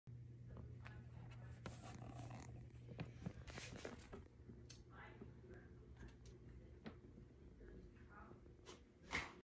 Nikko Cat Yawn and Stretch at the same time | Cat Shorts
Ragdoll Cat's Name:Nikko
Pls comment in this video